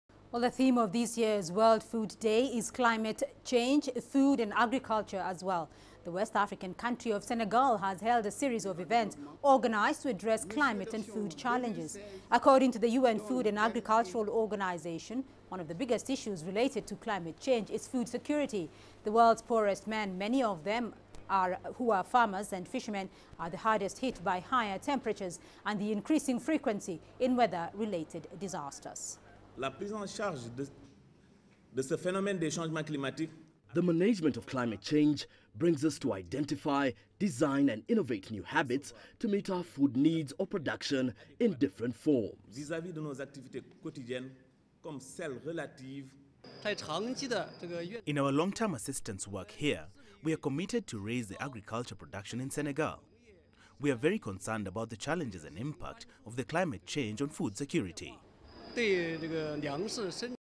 Senegal holds a series of events to address climate & food security challenges
The theme of this year's World Food Day is Climate change, food, and agriculture too. The west African country of Senegal has held a series of events organized to address climate and food challenges. According to the UN Food and Agriculture Organization, one of the biggest issues related to climate change is food security. The world's poorest men, many of whom are farmers and fishermen are the hardest hit by higher temperatures and the increasing frequency in weather-related disasters.